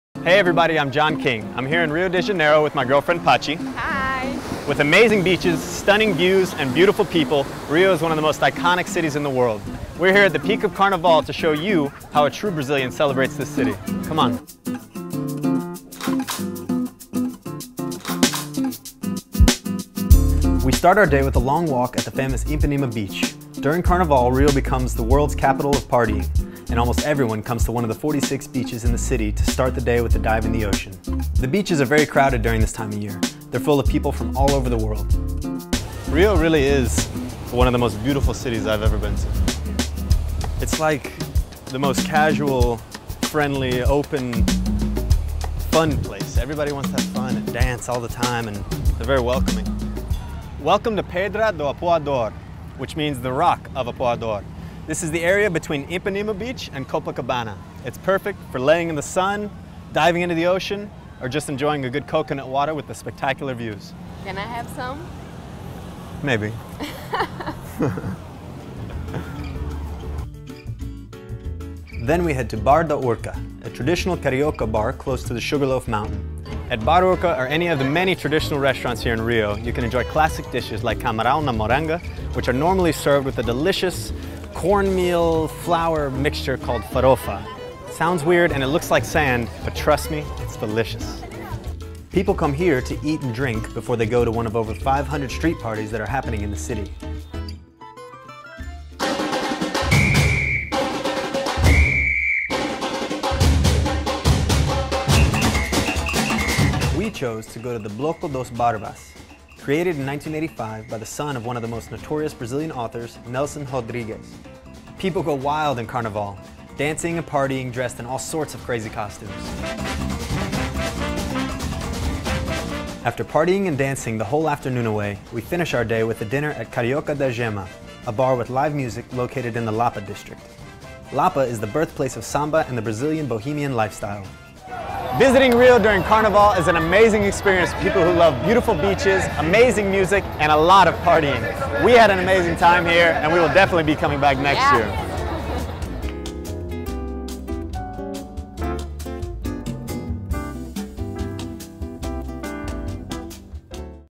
John King - Rio's Sexiest Hot Spots
Travel expert John King and his girlfriend, Patricia Werle, take in the sights and sounds of Carnival as they share their favorite must-visit spots around Rio de Janerio. Join the duo as they soak up some sun on one of Rio’s 46 beaches, sip coconut water on top of Pedra do Arpoador, and dance the night away.

John.king0705